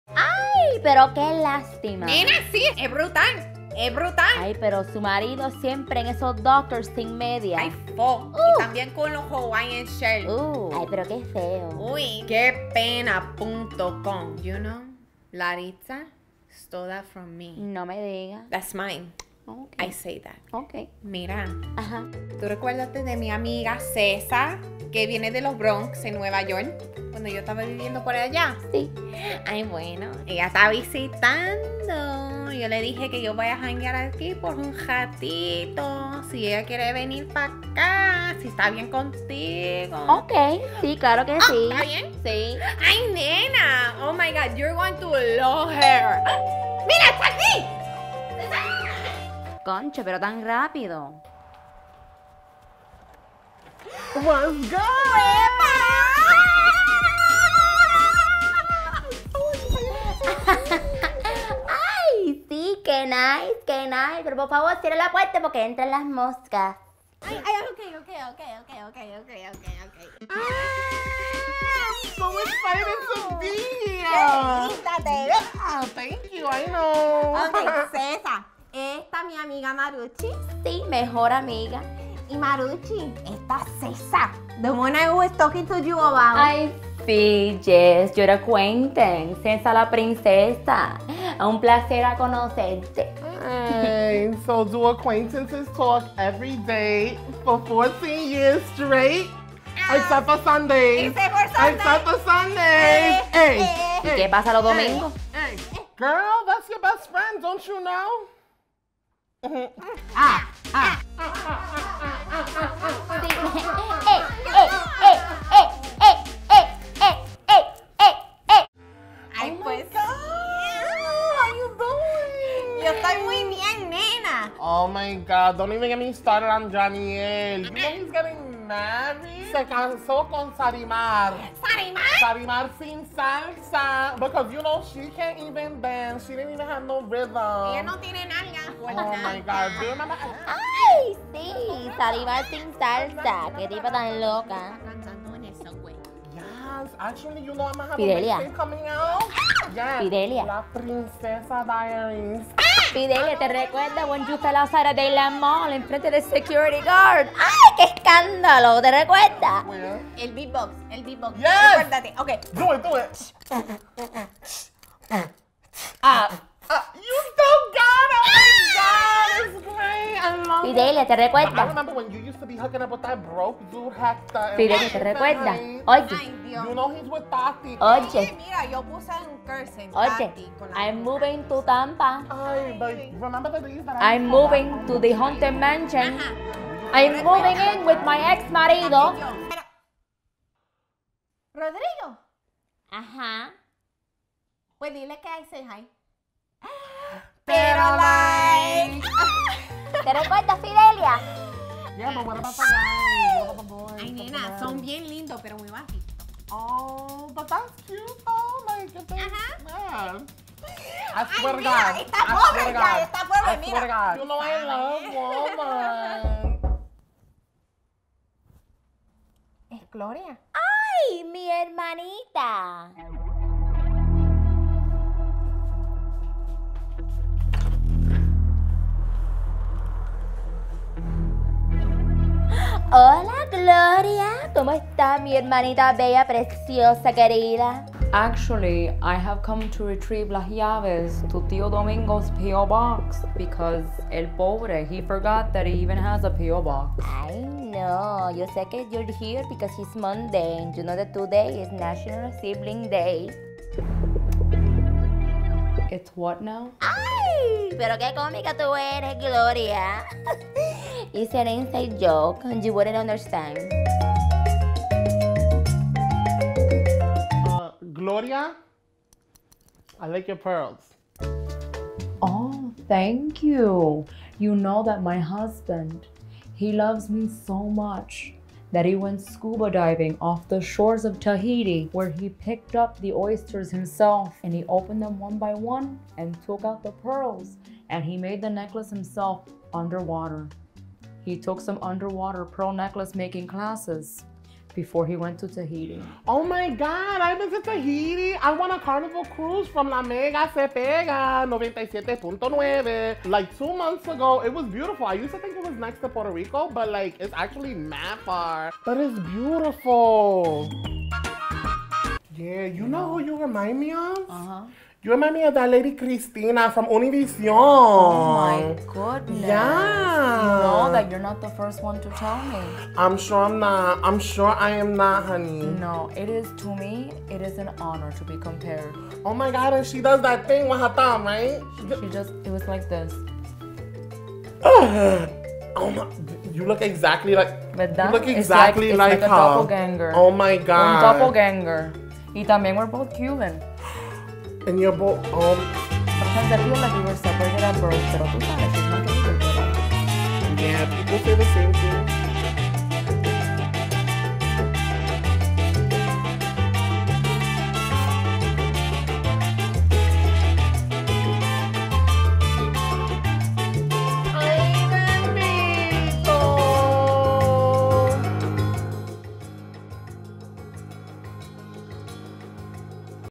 When Your Best Friend Has Another Best Friend
Sometimes you feel like you and your best friend are in a bubble of secrets, laughter, chisme y gozadera. Until you find out they have another best friend and you are shaken to your core. QUE ESCANDALO!

Guess we'll see what happens when Fidelia introduces Maruchi to her girl from the Bronx, Cessa!

Where's Jenny, meng?

[CREW]

Sound: Alex Breslau

[CAST]